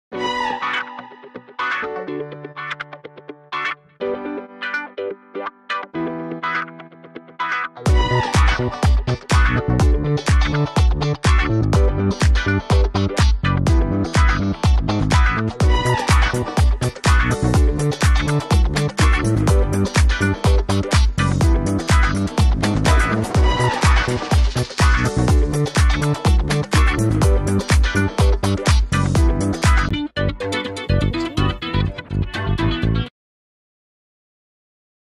Meet the B Pups!
This is the trailer and beginning of the B Pups! Here are the personality's of my dogs! Bo and Bandit.